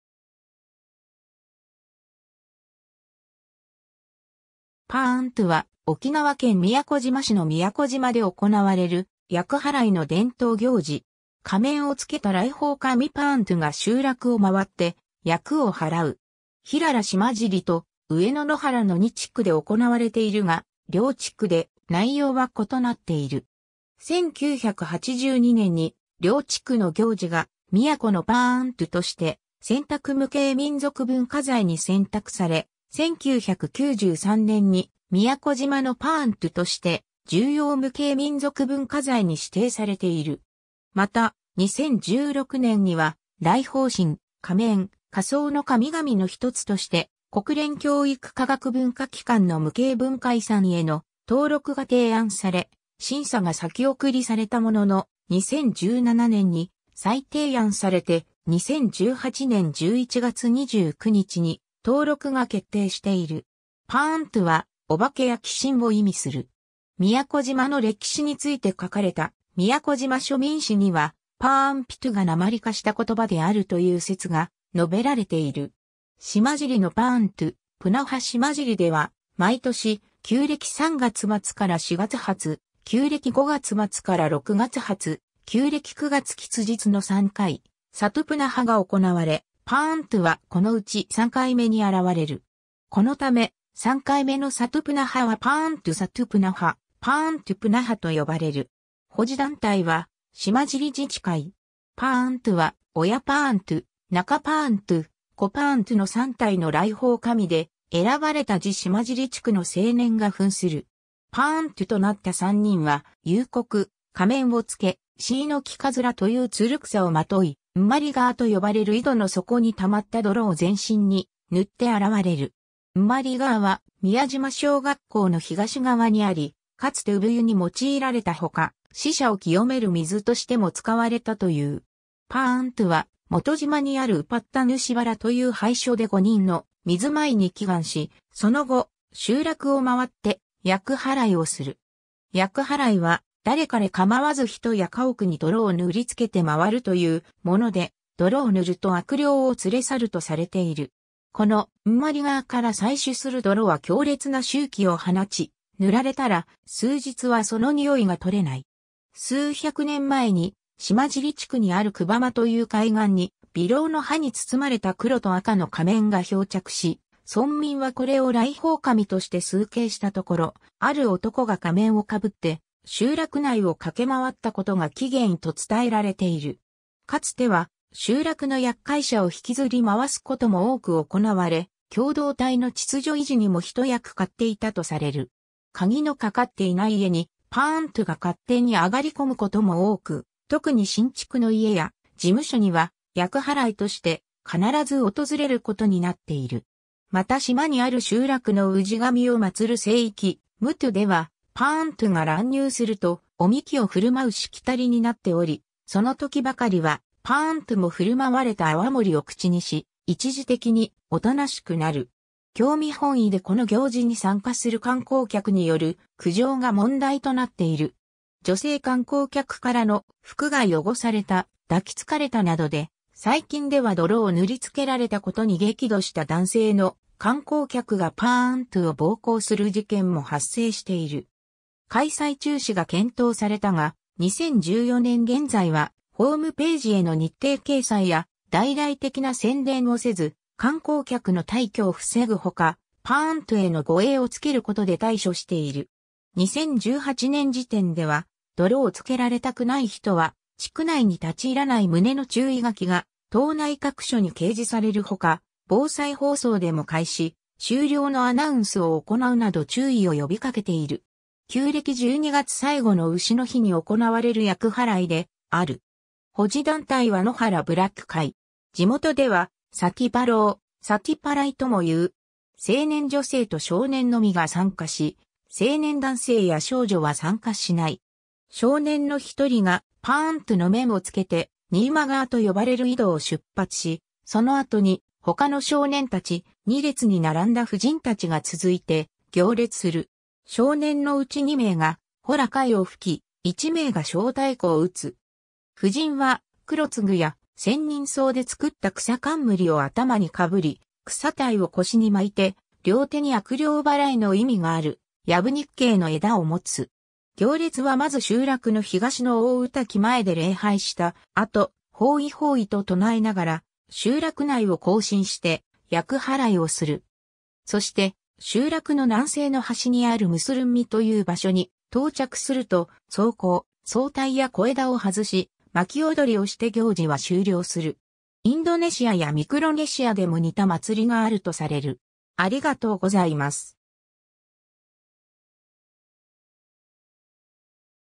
パーントゥ
来訪神：仮面・仮装の神々
パーントゥは、沖縄県宮古島市の宮古島で行われる厄払いの伝統行事。
仮面をつけた来訪神パーントゥが集落を回って厄をはらう。
平良島尻と上野野原の2地区で行われているが、両地区で内容は異なっている。
 1982年に両地区の行事が「宮古のパーントゥ」として選択無形民俗文化財に選択され、1993年に「宮古島のパーントゥ」として重要無形民俗文化財に指定されている。
また、2016年には「来訪神：仮面・仮装の神々」のひとつとして国連教育科学文化機関(UNESCO)の無形文化遺産への登録が提案され、審査が先送りされたものの、2017年に再提案されて、2018年11月29日に登録が決定している。
 「パーントゥ」は、お化けや鬼神を意味する。
宮古島の歴史について書かれた『宮古島庶民史』（稲村賢敷、1948年）には、「パーン（食む）+ピトゥ（人）」が訛化した言葉であるという説が述べられている。
 「島尻のパーントゥ・プナハ」（宮古島市総合博物館） 島尻では、毎年、旧暦3月末から4月初、旧暦5月末から6月初、旧暦9月吉日）の3回、サトゥプナハ（里願い）が行われ、パーントゥはこのうち3回目に現れる。
このため、3回目のサトゥプナハはパーントゥ・サトゥプナハ、パーントゥ・プナハと呼ばれる。
保持団体は島尻自治会。
 パーントゥは親（ウヤ）パーントゥ、中（ナカ）パーントゥ、子（フファ）パーントゥの3体の来訪神で、選ばれた字島尻地区の青年が扮する。
パーントゥとなった3人は、夕刻、仮面を着けシイノキカズラ（方言名：キャーン）という蔓草をまとい、「ンマリガー」（産まれ泉）と呼ばれる井戸の底に溜まった泥を全身に塗って現れる。
「ンマリガー」は、宮島小学校（2017年閉校）の東側にあり、かつて産湯に用いられたほか、死者を清める水としても使われたという。
パーントゥは、元島（集落発祥の地）にあるウパッタヌシバラという拝所で5人のミズマイ（神女）に祈願し、その後、集落を回って厄払いをする。
厄払いは誰彼かまわず人や家屋に泥を塗りつけて回るというもので、泥を塗ると悪霊を連れ去るとされている。
この「ンマリガー」から採取する泥は強烈な臭気を放ち、塗られたら数日はその臭いが取れない。
 数百年前に、島尻地区にあるクバマ（クバ浜）という海岸にビロウ（方言名：クバ）の葉に包まれた黒と赤の仮面が漂着し、村民はこれを来訪神として崇敬したところ、ある男が仮面をかぶって集落内を駆け回ったことが起源と伝えられている。
かつては集落の厄介者を引きずり回すことも多く行われ、共同体の秩序維持にも一役買っていたとされる。
 鍵のかかっていない家にパーントゥが勝手に上がり込むことも多く（当然家の中は泥だらけになる）、特に新築の家や事務所には厄払いとして必ず訪れることになっている。
また島にある集落の氏神を祀る聖域「ムトゥ」では、パーントゥが乱入するとお神酒を振る舞うしきたりになっており、そのときばかりはパーントゥも振る舞われた泡盛を口にし、一時的に大人しくなる。
 興味本位でこの行事に参加する観光客による苦情が問題となっている。
女性観光客からの「服が汚された」「抱きつかれた」などで、最近では泥を塗りつけられたことに激怒した男性の観光客がパーントゥを暴行する事件も発生している。
開催中止が検討されたが、2014年現在は、ホームページへの日程掲載や大々的な宣伝をせず、観光客の大挙を防ぐほか、パーントゥへの護衛を付けることで対処している。
2018年時点では、「泥をつけられたくない人は地区内に立ち入らない」旨の注意書きが島内各所に掲示されるほか、防災放送でも開始・終了のアナウンスを行うなど注意を呼びかけている。
 旧暦12月最後の丑（うし）の日に行われる厄払いである。
保持団体は野原部落会。
 地元ではサティパロウ、サティパライ（里祓い、さとばらい）ともいう。
成年女性と少年のみが参加し、成年男性や少女は参加しない。
少年のひとりがパーントゥの面を着けて「ニーマガー」と呼ばれる井戸を出発し、その後に他の少年達、2列に並んだ婦人達が続いて行列する。
少年のうち2名がほら貝を吹き、1名が小太鼓を打つ。
婦人はクロツグ（方言名：マーニ）やセンニンソウ（方言名：タドゥナイ）で作った草冠を頭に被り、草帯を腰に巻いて、両手に悪霊祓いの意味があるヤブニッケイ（方言名：ツッザギー）の枝を持つ。
行列はまず集落の東の大御嶽前で礼拝した後、「ホーイホーイ」と唱えなが...